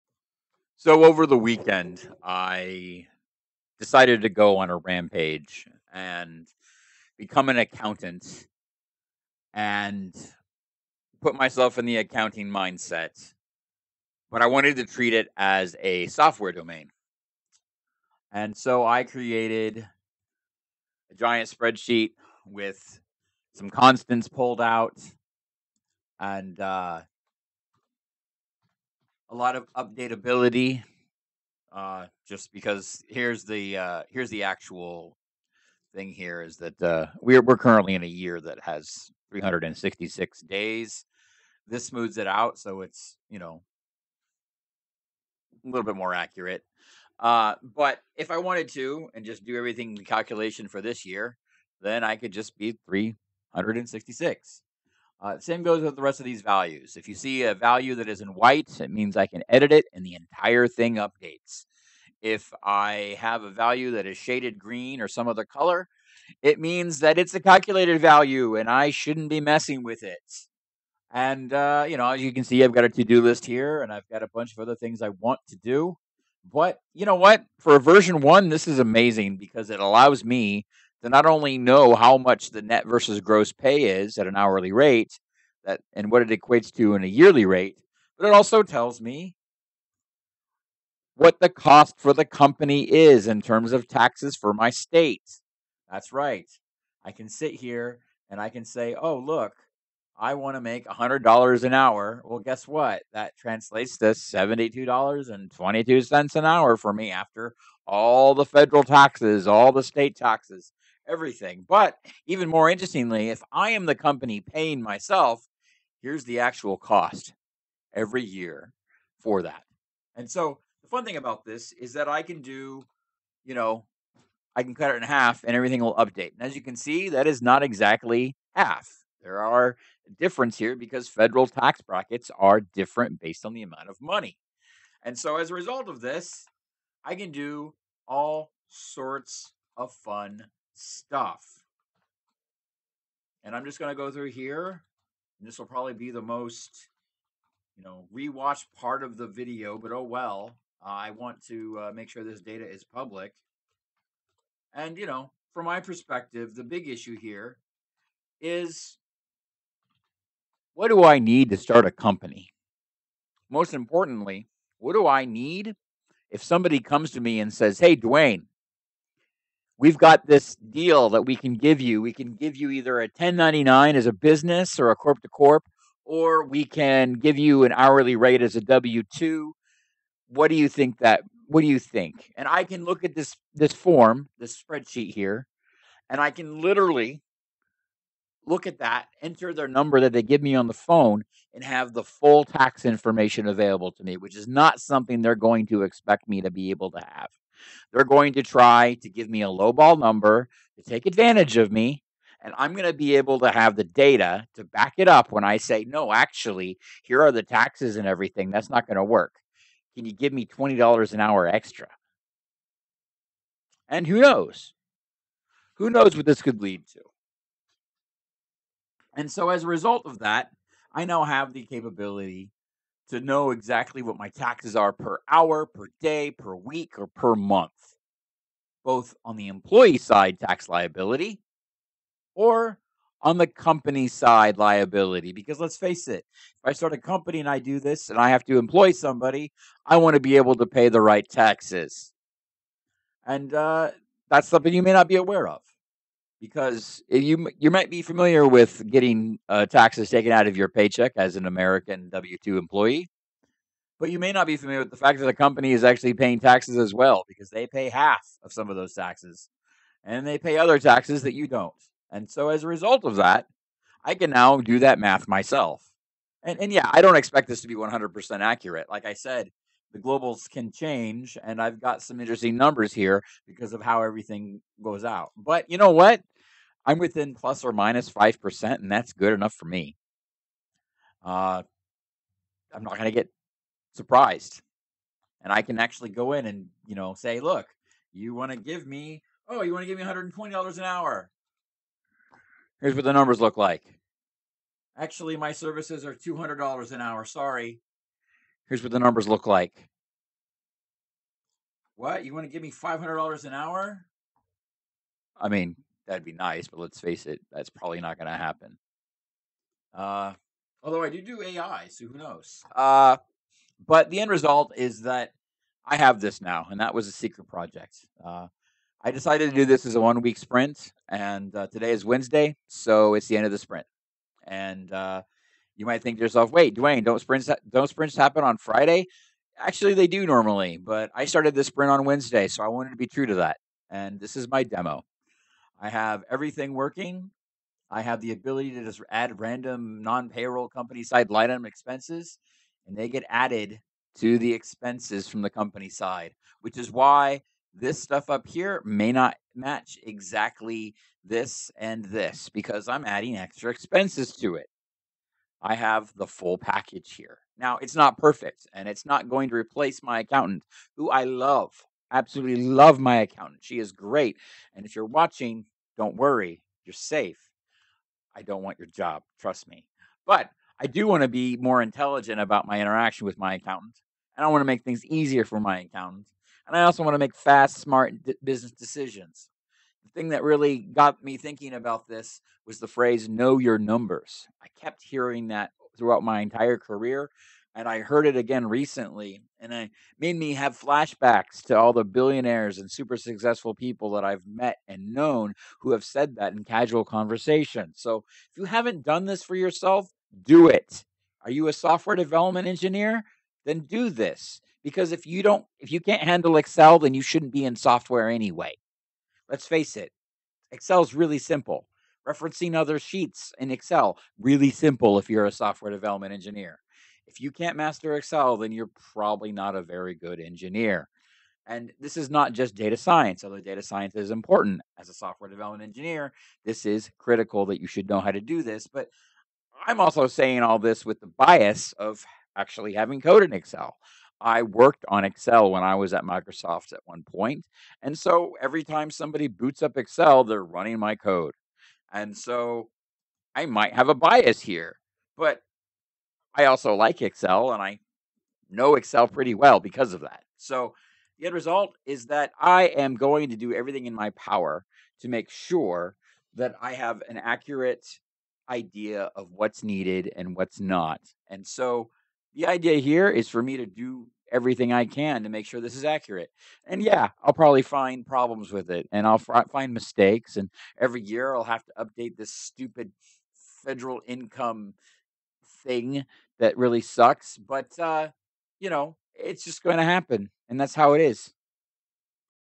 Project Demo: Know Your Numbers
If you do not have one, get an accountant. If you have one, try to do their job for a week, just so you have greater empathy for what they do.

As a software engineer, you should not be afraid of Excel or Data Science.

It's a common business meme that people should know their numbers.

So I went into raging accountant mode (not like that) over the week, ad built up an excel spreadsheet to ballpark some numbers.

This is my Wednesday project demo, because I got the idea for this last Wednesday. So this was a 1 week sprint, with this not being my primary focus for the week, just a side project.